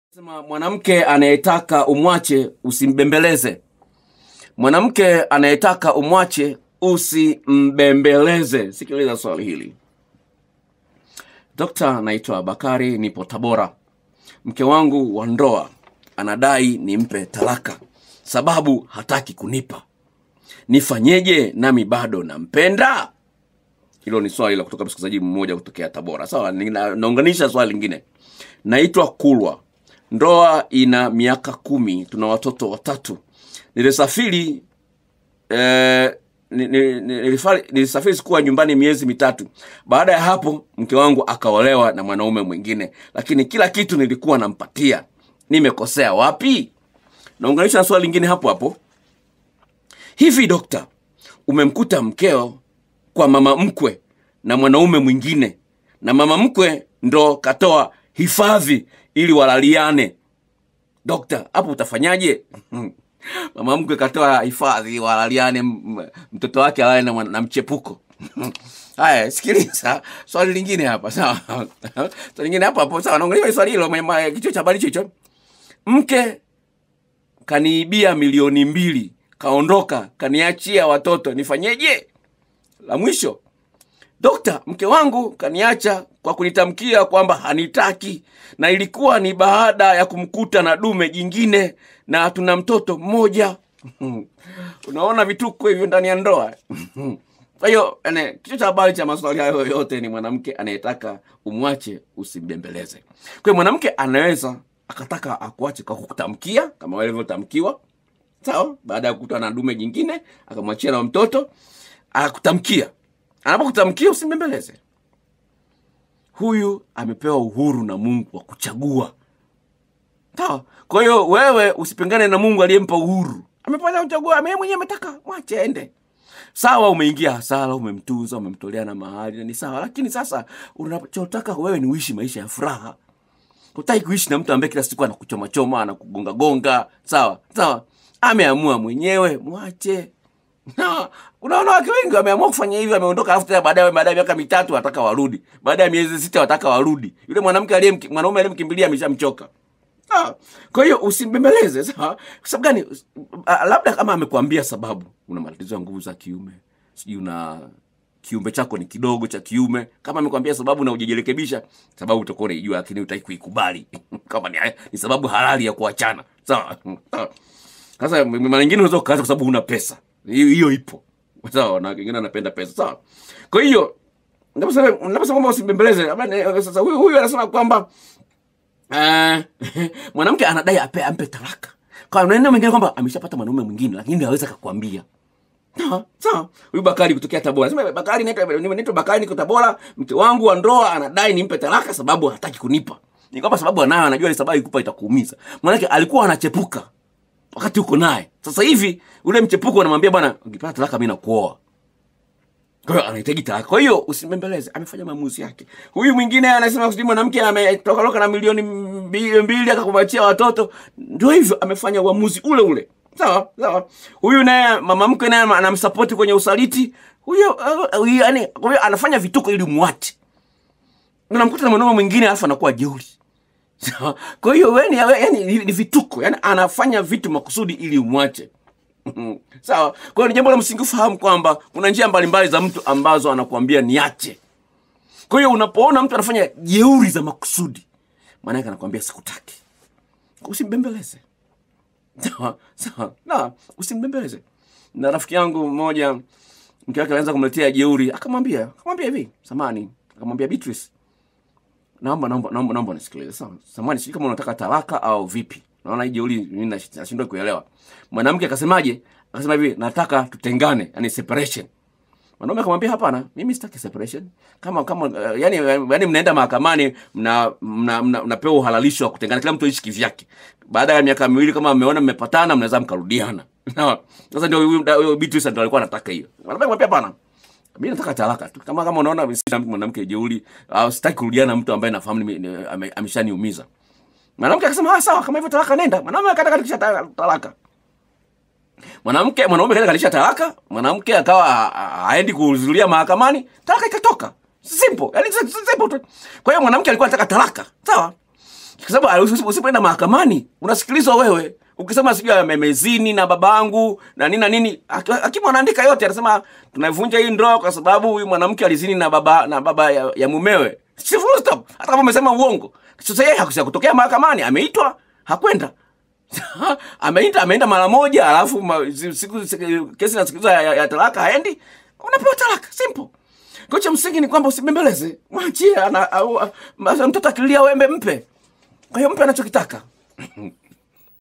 Mwanamke anaetaka umuache usimbembeleze usidhanie anakupima tu
Kila mmoja anao uhuru wa kuchagua na ni haki yake kuchagua njia aitakoyo katika maisha yake